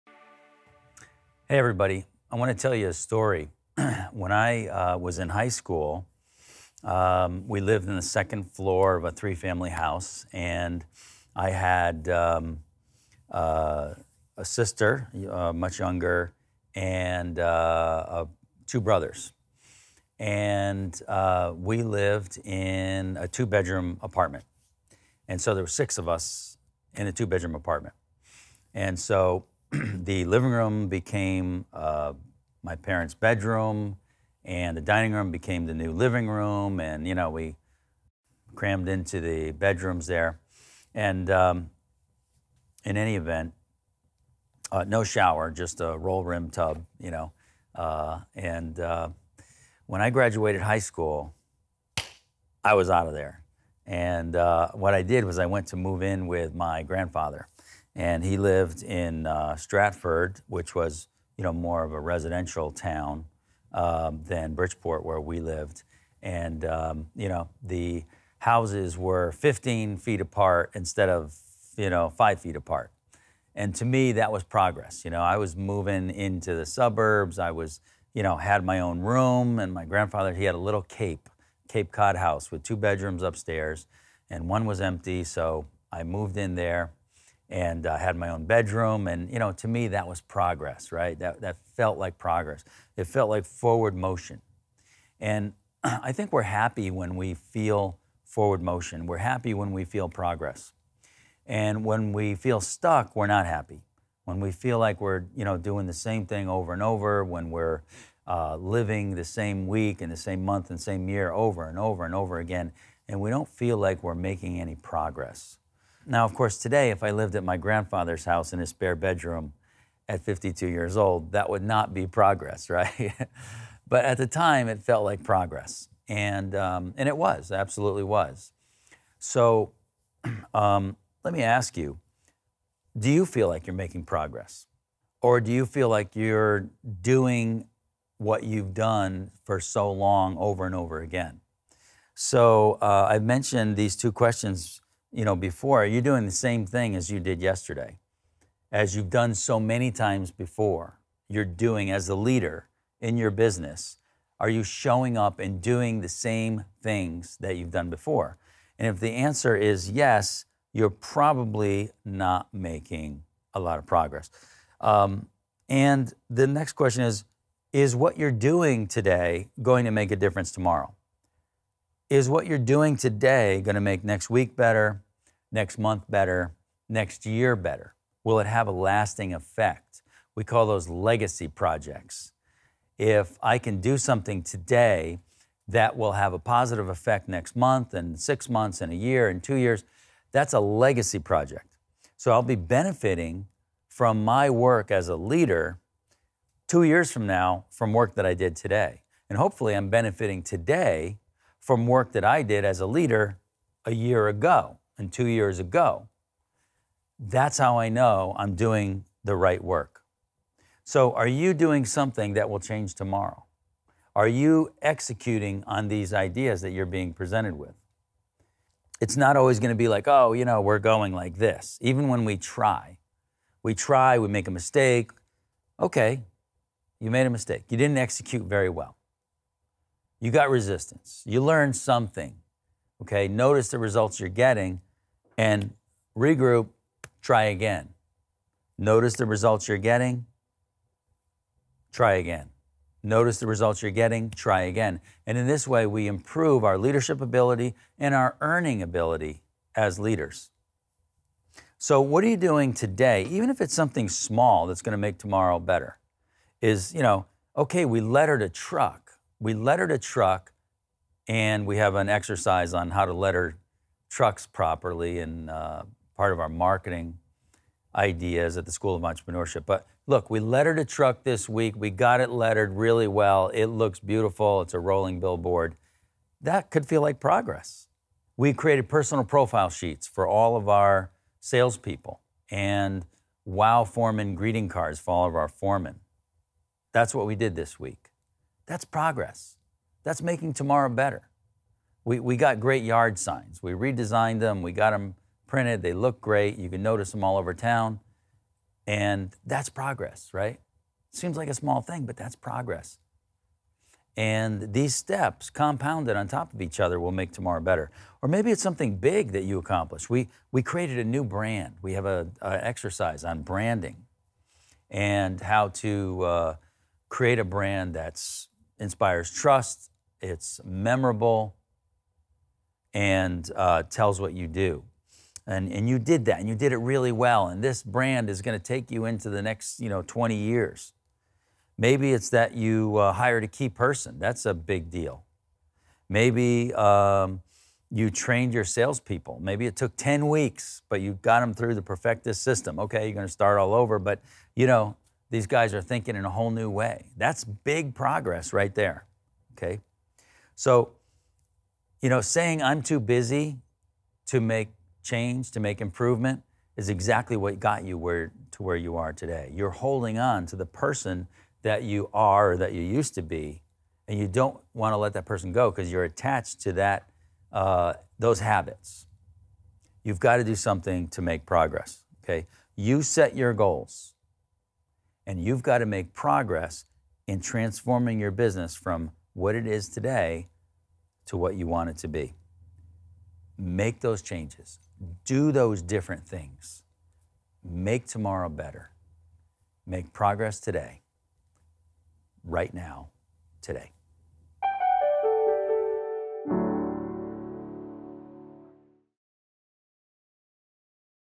Stuck or Growing? It's Up to You | Think Daily for Business
What you work on today makes all the difference to your business tomorrow.

Want to relive the same day over and over? Keep doing the same things day after day.

Or do you want to improve your earning ability and leadership ability? It's up to you and what you choose to work on.

Watch the video then head over to The School of Entrepreneurship and learn exactly what you should be working on.